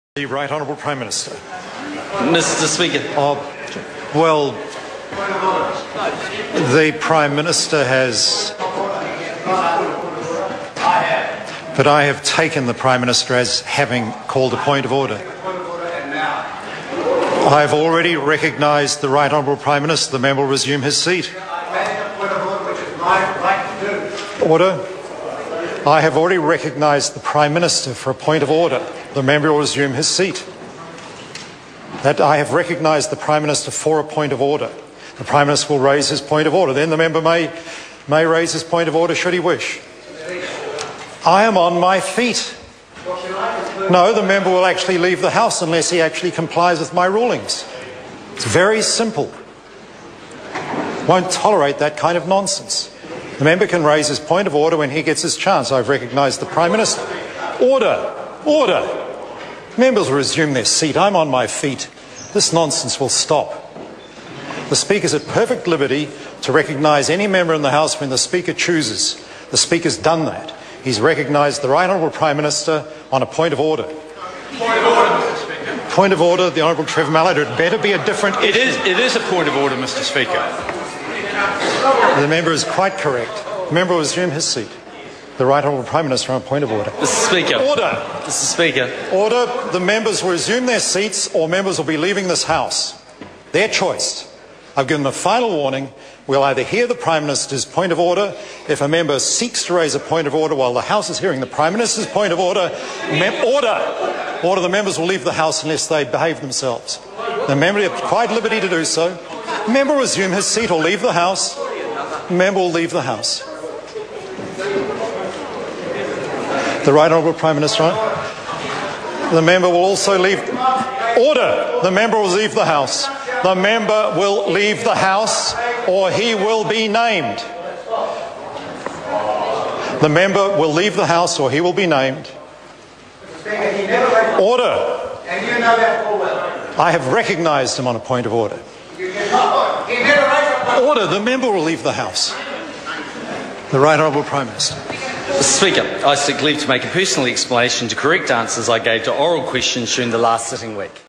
Speaker Claims Two Scalps
Speaker Lockwood Smith ejects Labour's David Parker & NZ First Leader Winston Peters from the House after they refuse to cease raising points of order. The incident happened after the Speaker had already called on a Point of Order from the Prime Minister.